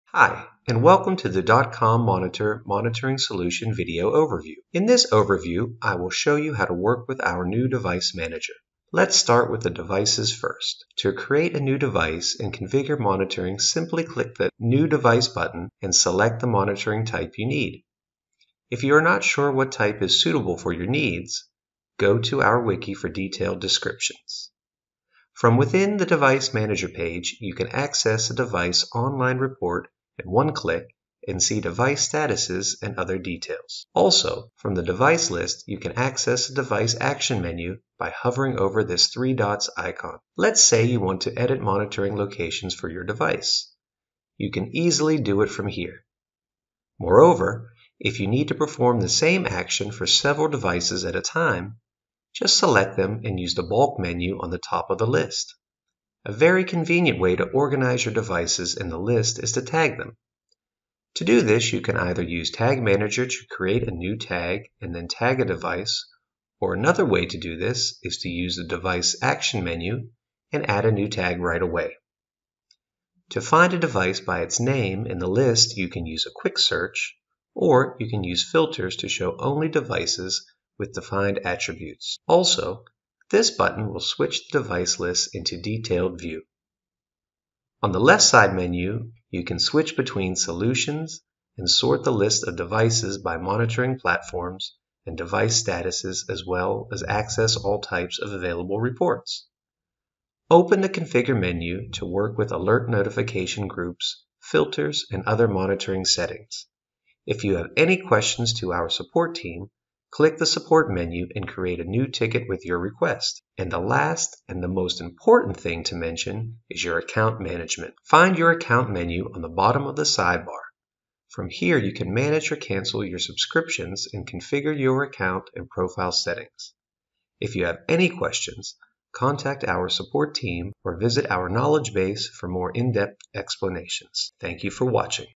Dotcom-Monitor Device Manager
Learn more about managing your monitoring devices with the Device Manager within the Dotcom-Monitor platform. View specific devices details, like the number and current status of each device, monitoring frequency, and the timestamp the device was last monitored. You can also carry out several actions, such as enabling, postponing, and silencing a device, as well as cloning a device or task, deleting a device or task, and running a status report or an SLA report.